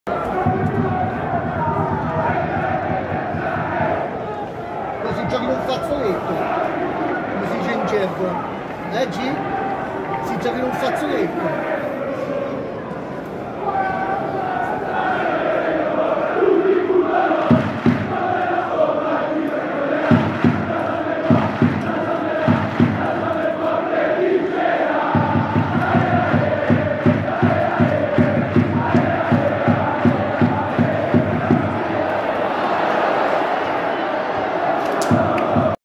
Sambenedettese- L'Aquila: i tifosi rossoblu allo stadio
S.Benedetto del Tronto, 26 novembre 2023